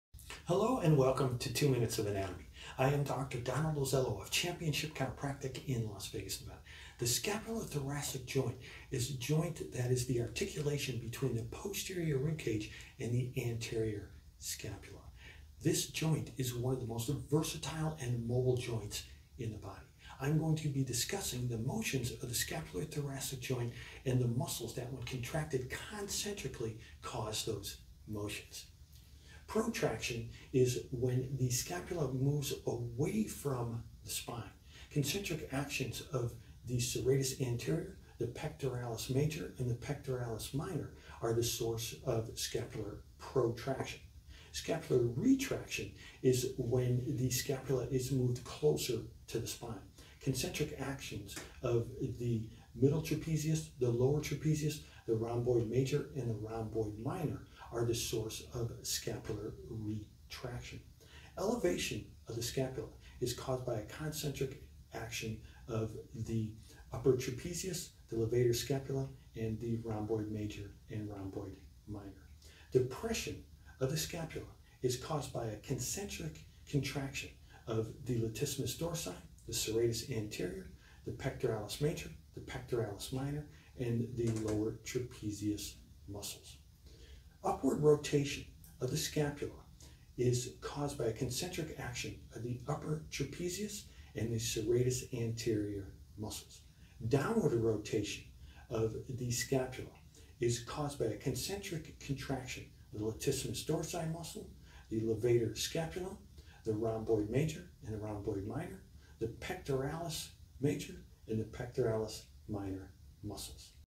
Two Minutes of Anatomy: Scapulothoracic Joint
Scapulothoracic Joint: Articulation between posterior ribcage and anterior scapula. One of the most mobile & versatile joints.
Protraction: Serratus anterior, pectoralis major & pectoralis minor.
Retraction: Middle trapezius.lower trapezius, rhomboid minor & rhomboid major.
Elevation: Upper trapezius, levator scapulae & rhomboids.
Depression: Latissimus dorsi, serratus anterior, pectoralis major, pectoralis minor & lower trapezius.
Upward Rotation: Trapezius & serratus anterior.
Downward Rotation: Latissimus dorsi, levator scapulae, rhomboids, pectoralis major & pectoralis minor muscles.